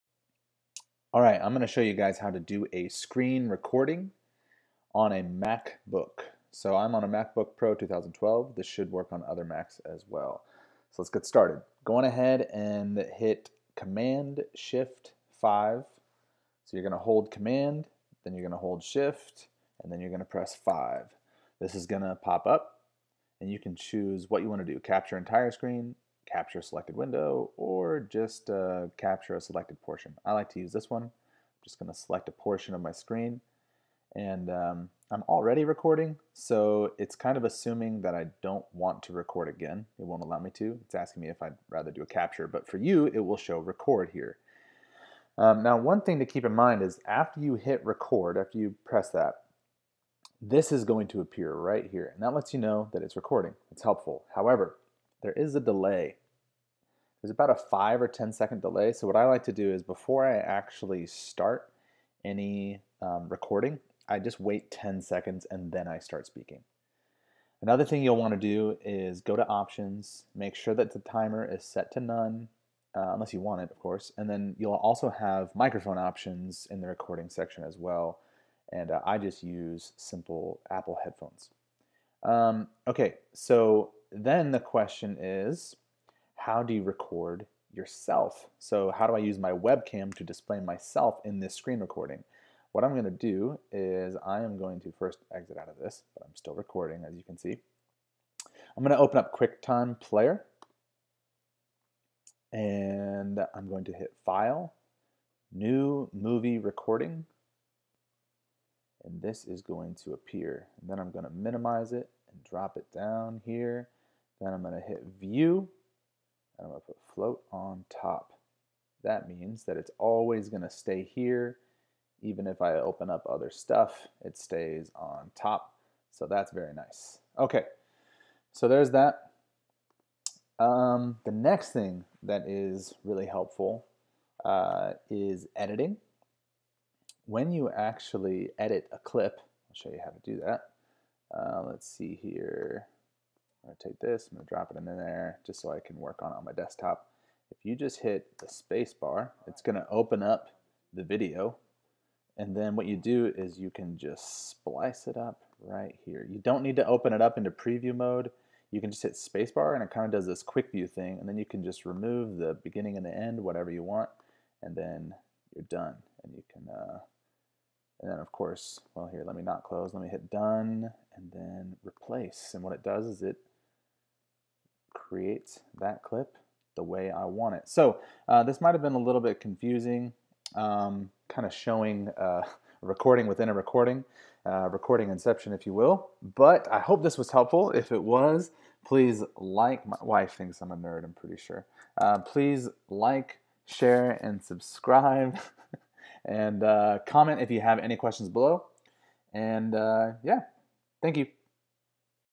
How to Do a Screen Recording on a MacBook
In this video, I show you how to record your screen while simultaneously recording yourself through your webcam, as well as how to quickly and easily edit the recording, on a MacBook Pro 2012. This same method should work for other Macs as well.